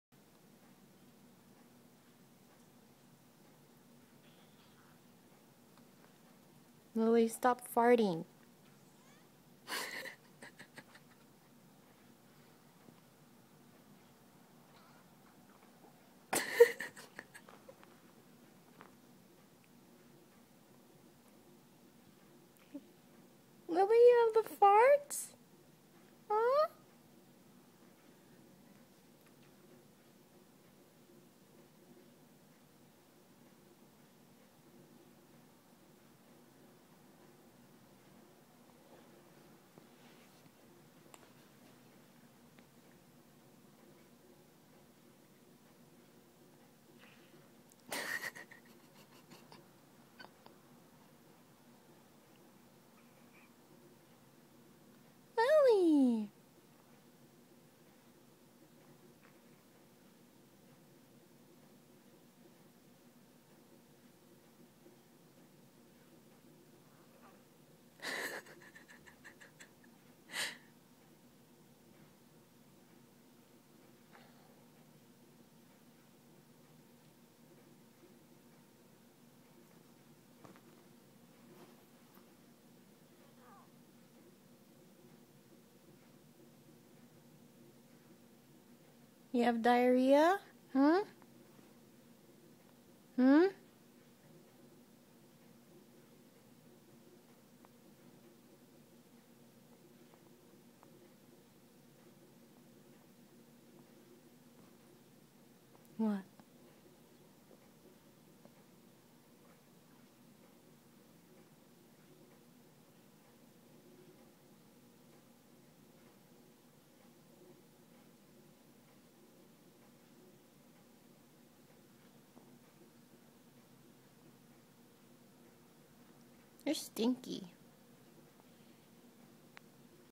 Lily Has the Farts
I was watching tv with my dog, Lily, on the armrest when I kept hearing her tummy grumble. I suddenly smelled something rancid and realized it was actually her farts.